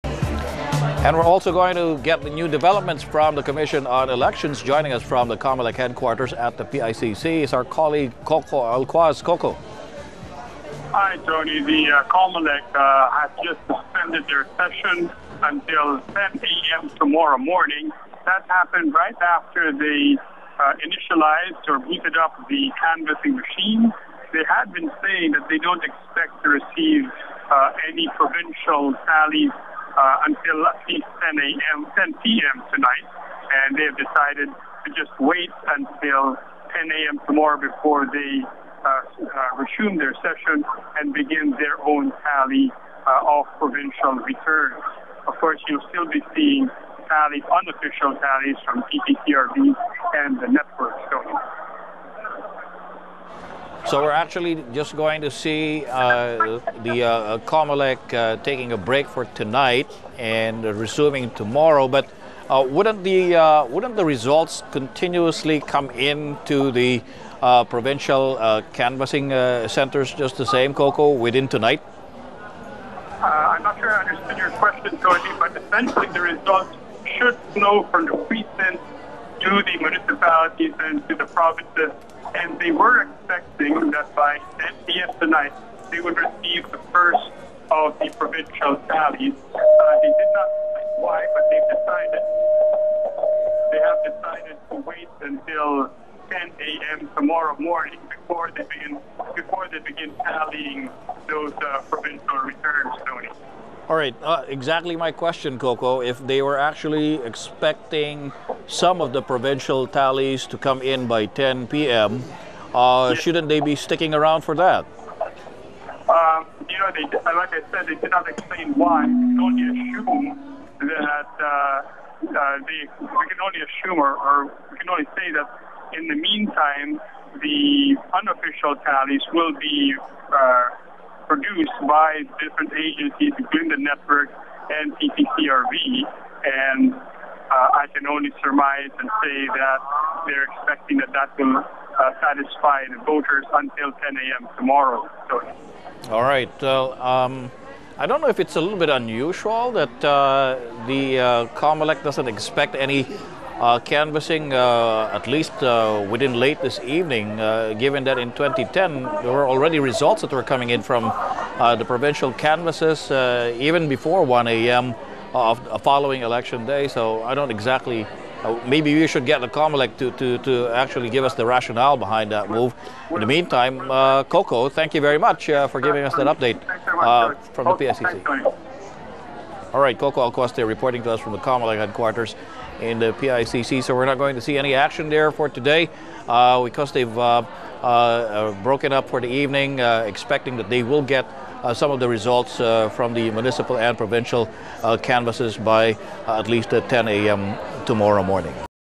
Comelec to reconvene as national board of canvassers Tuesday morning
Coco Alcuaz reports from the Comelec